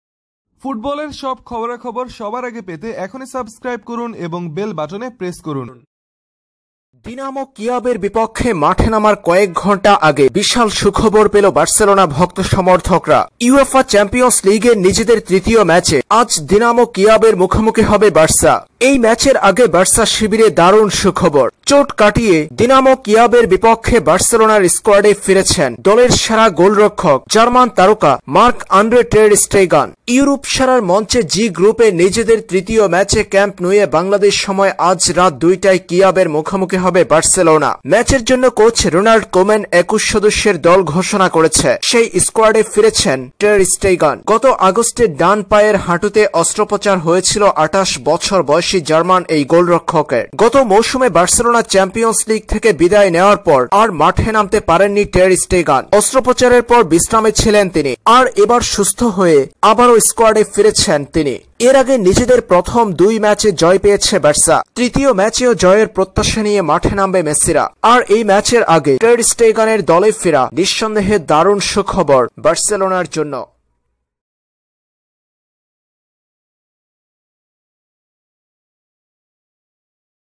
সুখবর! মাঠে নামার কয়েক ঘণ্টা আগেই যে বিশাল সুখবর পেল বার্সা! আনন্দে ভাসছেন মেসি | fcb vs dynamo
বিস্তারিত জানুন।